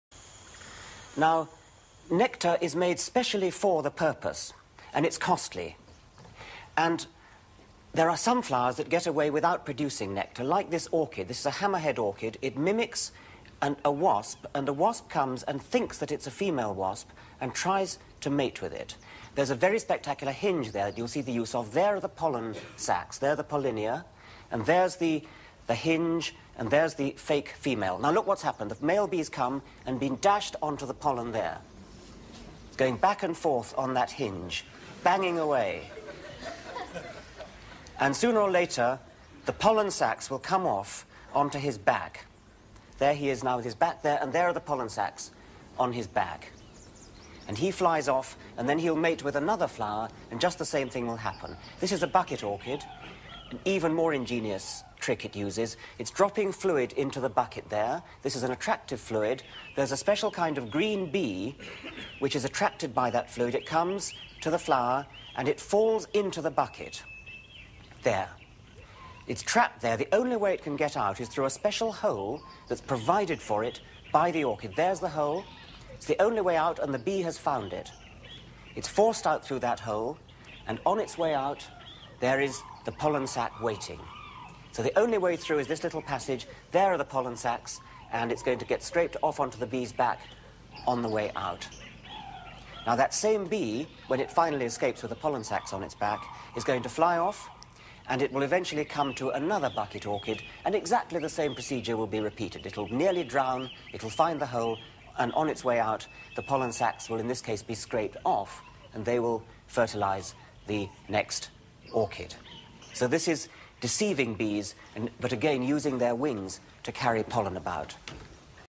Orchidee reproduction Richard Dawkins bees and deceitful plants 1991
Mode de reproduction des orchidée dont l'orchidée marteau qui attire le mâle d'une espèce d'une guêpe en émettant les mêmes phéromones (odeur hormonale émise par la femelle pour attirer les mâles). Le mâle cherche à se reproduire avec la fleur. Pour cela il actionne un mécanisme du type marteau enclume En heurtant la partie "enclume" de la d fleur celle ci lui donne alors ses sacs polliniques contenant le pollen. En visitant une autre fleur, le mâle fécondera une autre fleur d'orchidée marteau.
L'extrait est en anglais mais les images sont magnifiques.